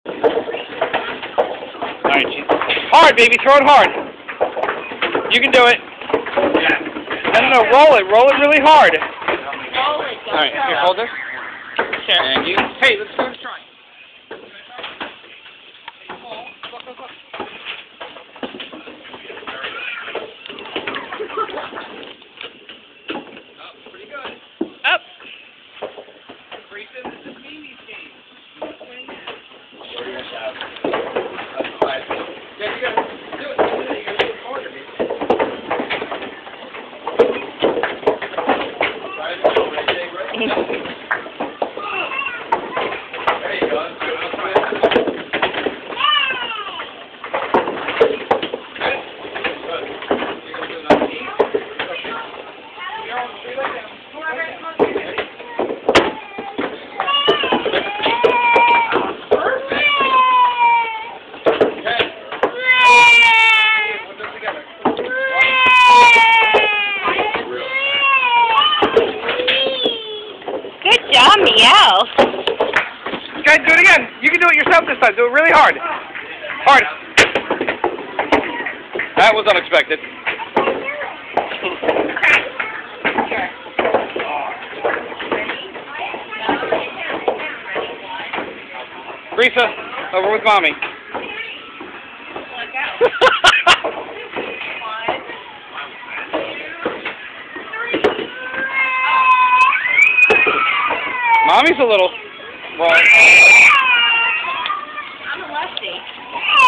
06-21-2009 - Miel Gabby and Brisa Rosania - Moonlight trip to Seaside Heights - 07 (Skee Ball)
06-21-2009 - Miel Gabby and Brisa Rosania - Moonlight trip to Seaside Heights - 07 (Skee Ball)